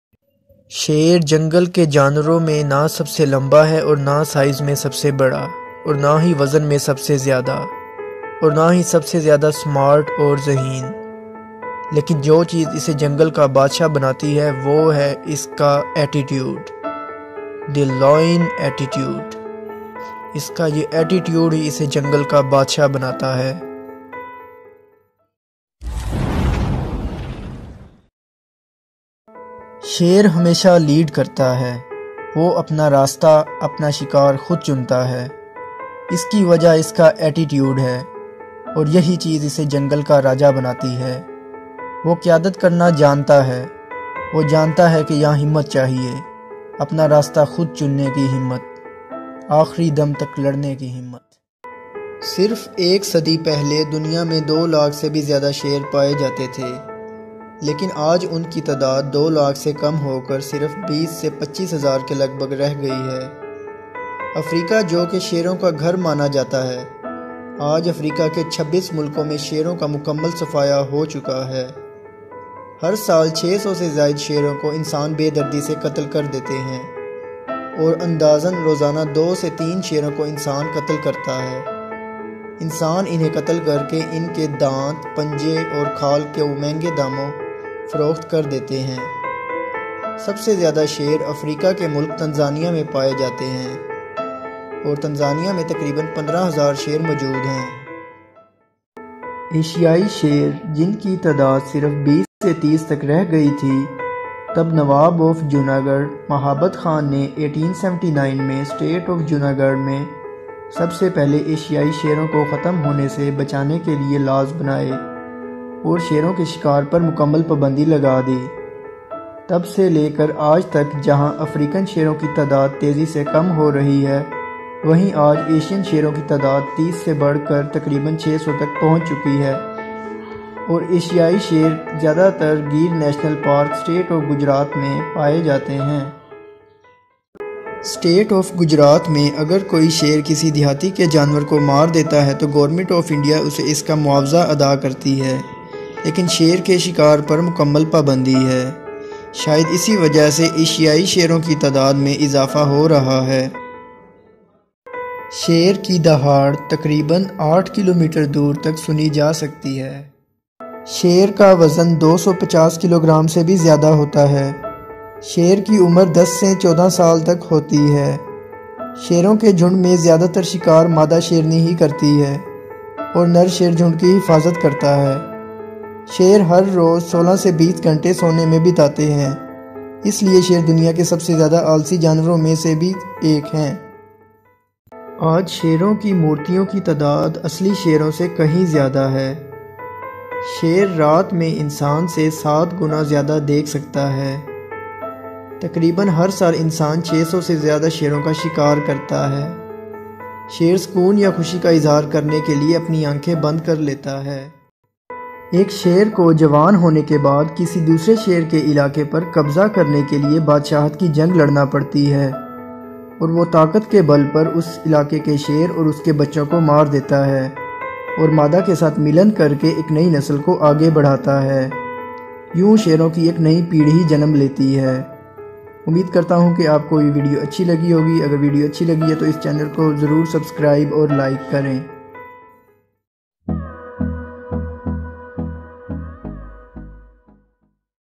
INTERESTING FACTS ABOUT LIONS| When only 20 Asian lions were left in the world😲| Umar Ashiq TV
When only 20 asain lions were left in the world. The battle for the king of jungle. African vs Asian lions. Lions in Tanzania. Lion roar.